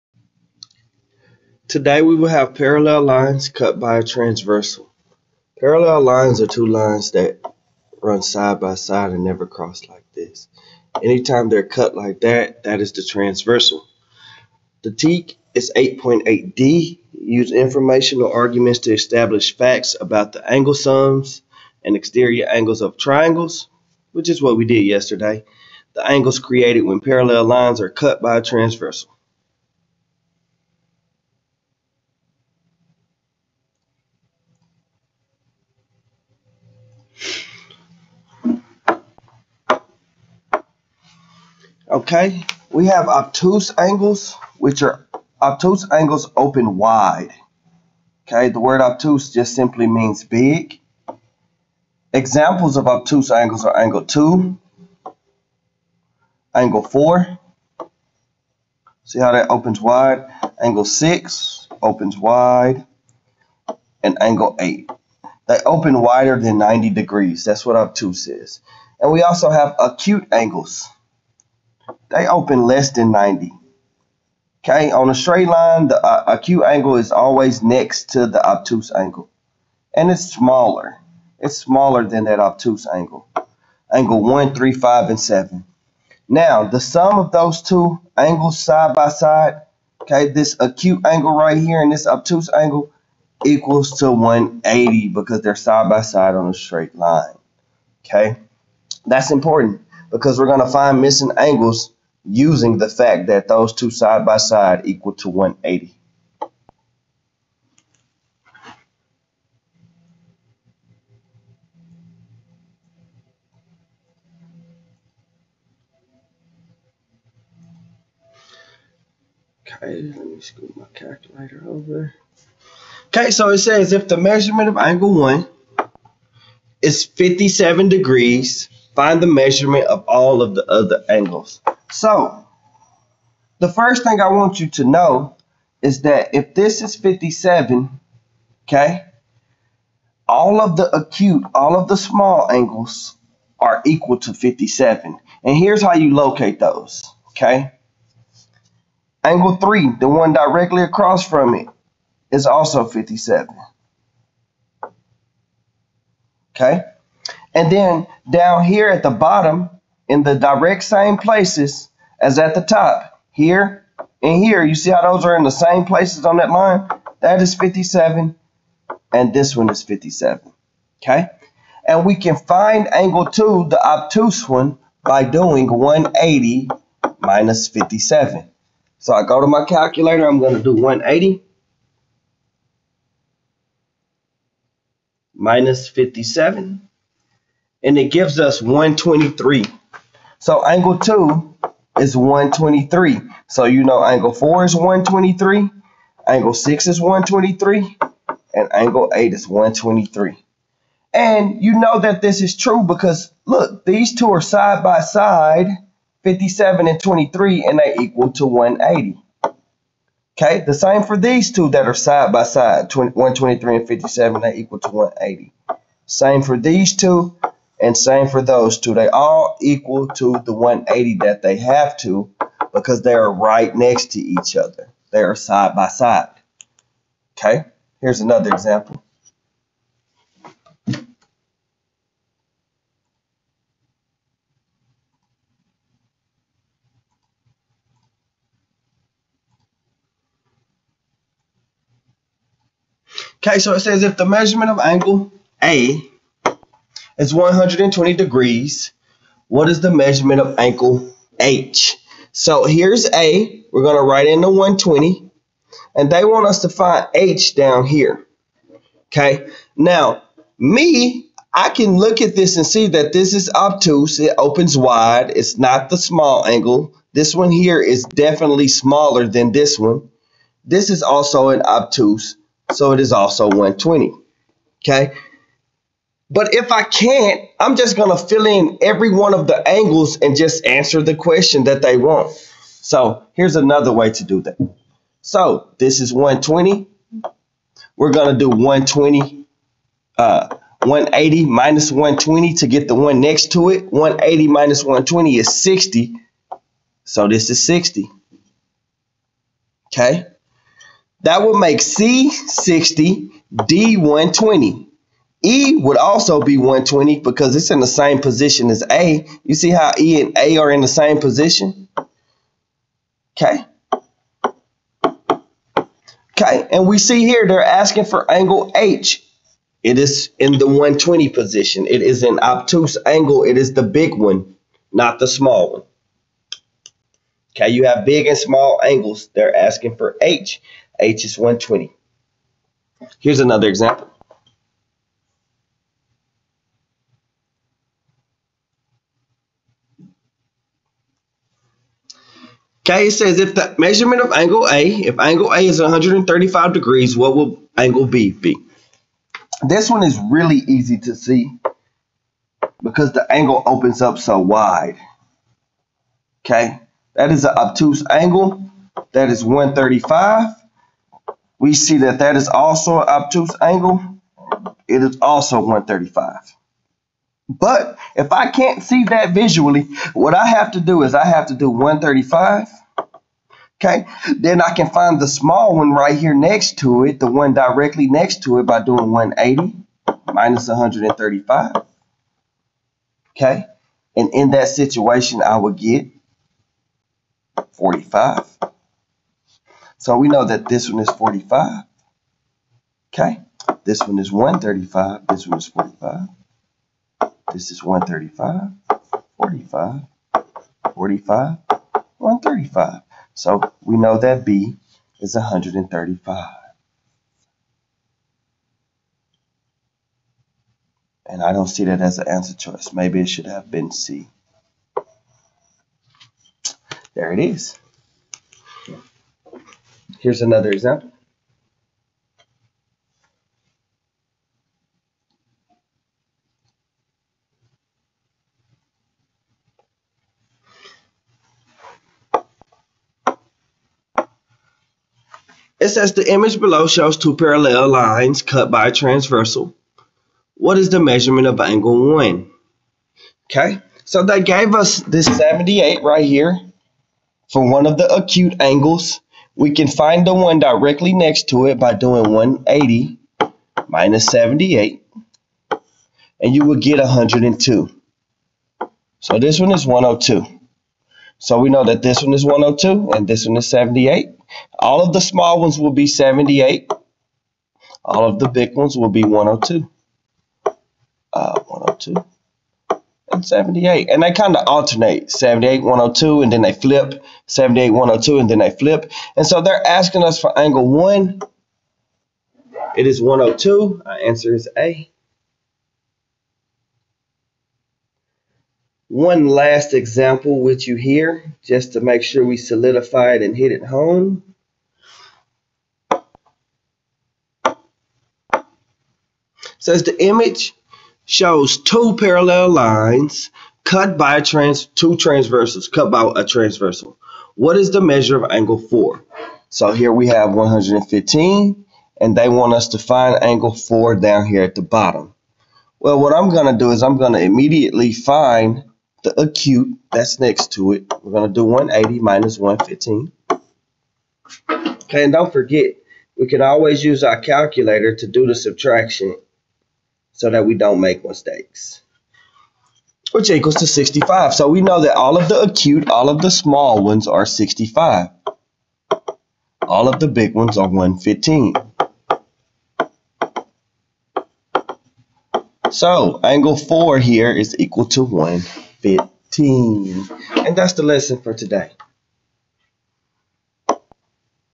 Parallels cut by transversal
8.8D – use informal arguments to establish facts about the angle sum and exterior angle of triangles, the angles created when parallel lines are cut by transversal, and the angle-angle criterion for similarity of triangles